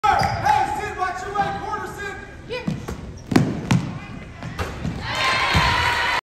Riley Allen knocks down the 3-point shot for Chatham vs. Morris Hills
Senior Riley Allen hits the 3-pointer for Chatham in its win vs. Morris Hills the first round of the Morris County Tournament